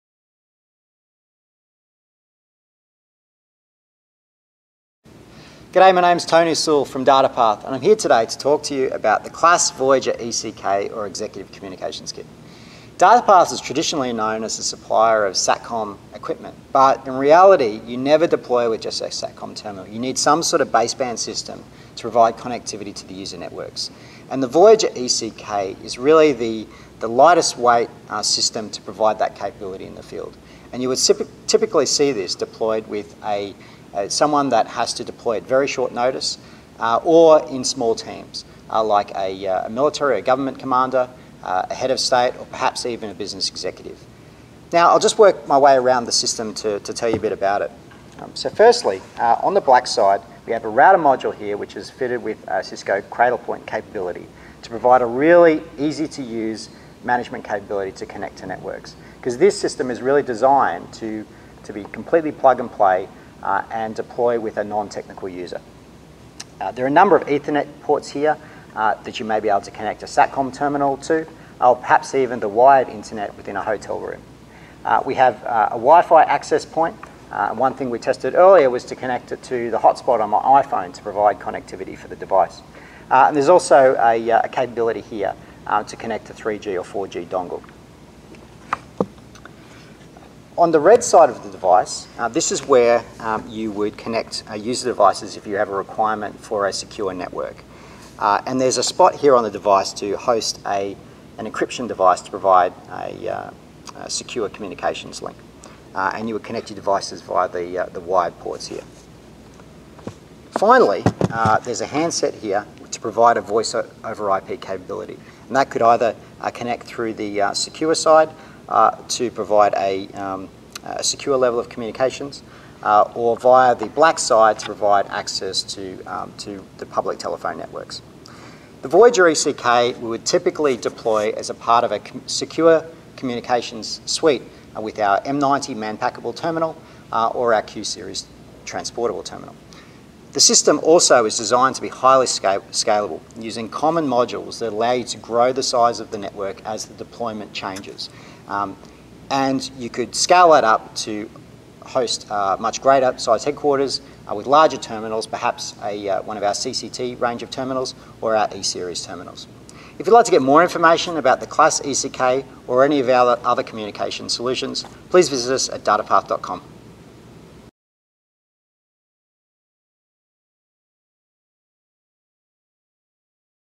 Voyager Executive Communications Kit Product Overview
The Voyager ECK (Executive Communications Kit) provides VoSIP and multi-enclave data capability in a small, portable, lightweight package. With its built-in cellular and Wi-Fi transport options, the Voyager ECK solution enables global access to secure network resources even when a user is on the move. Furthermore, Voyager ECK is easy to use with an average set up time of less than three minutes. Voyager ECK also meets a bring-your-own-device (BYOD) requirement by allowing users to utilize their existing laptop computer.